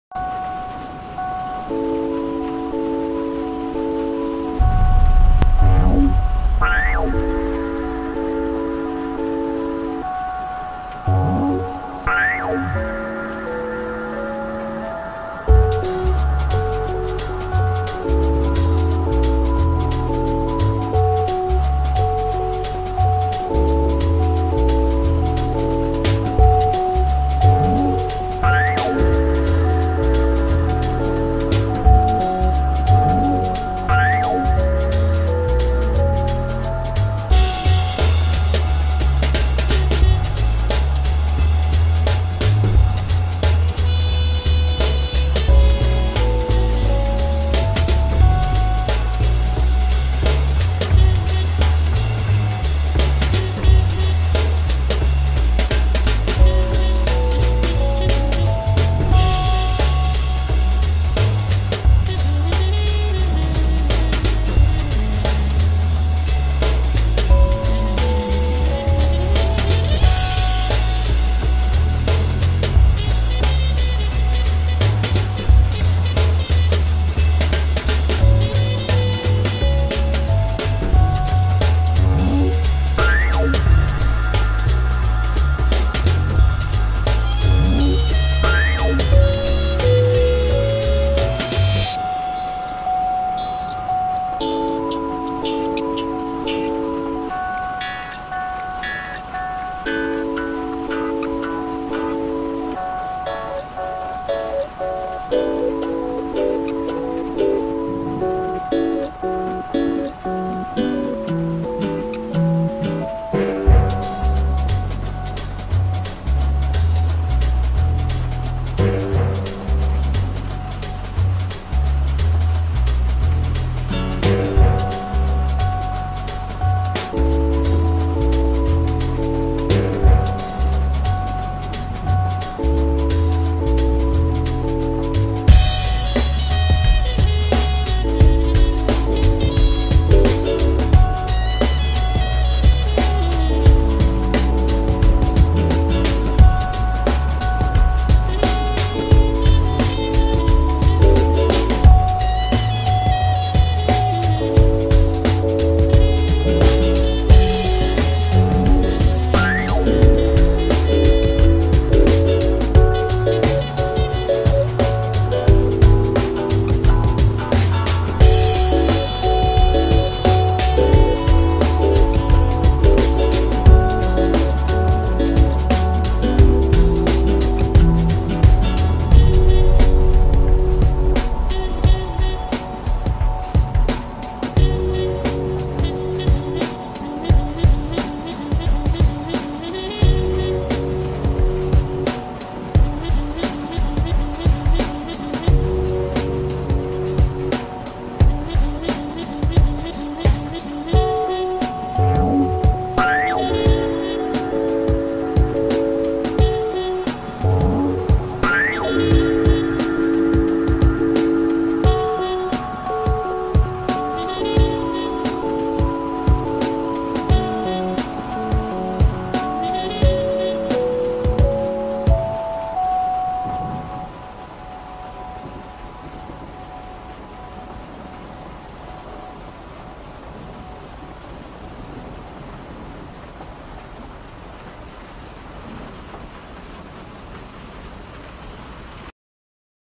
It's 3am and you're in Hey Arnold's room listening to jazz while it's raining | FREE MUSIC
It's 3am and you're in Hey Arnold's room listening to jazz while it's raining and you finally fell asleep.

This song is 100% copyright safe to use in your visual projects.